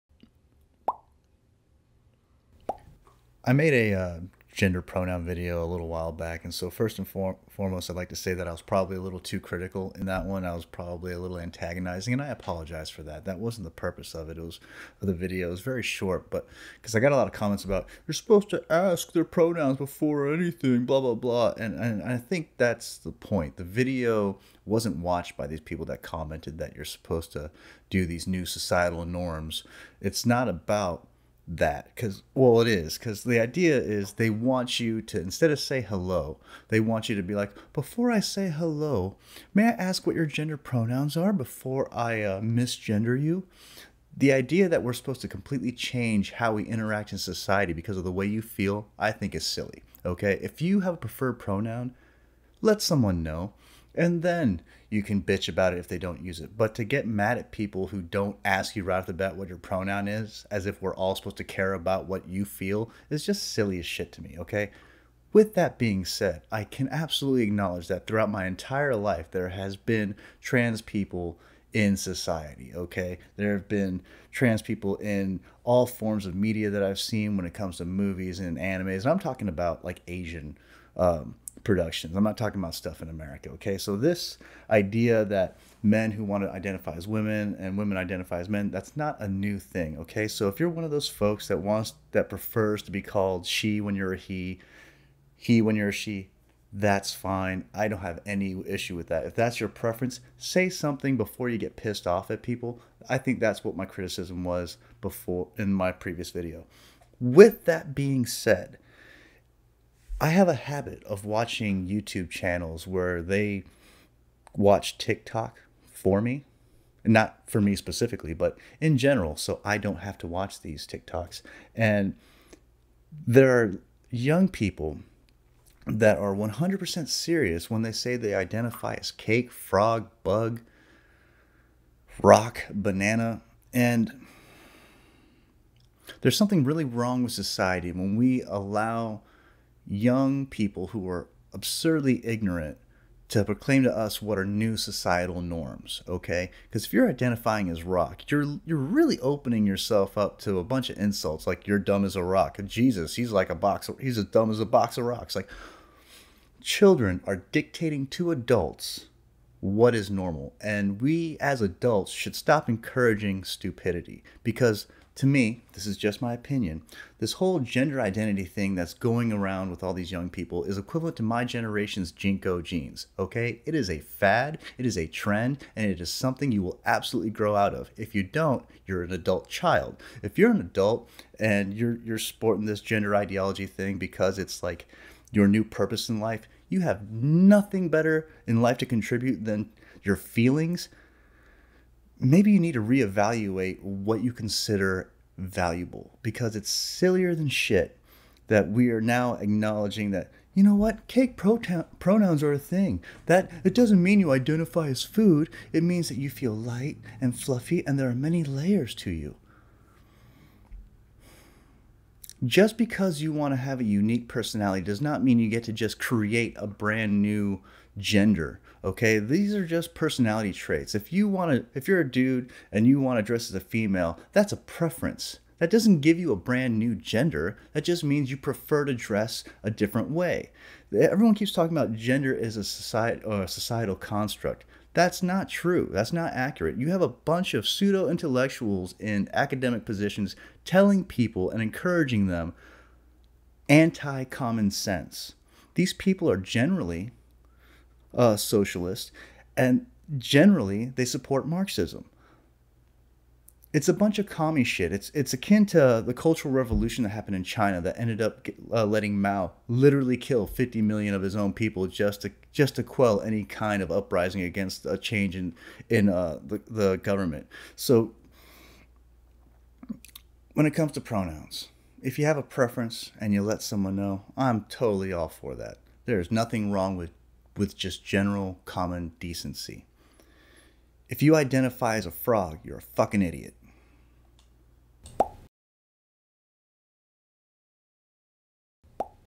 My bad about pronouns
I want to apologize for my last video on pronouns. I know what I said could be seen as antagonizing and that wasn't really my intention. With that being said...I will likely have to apologize for this video at some point. meh...fuck it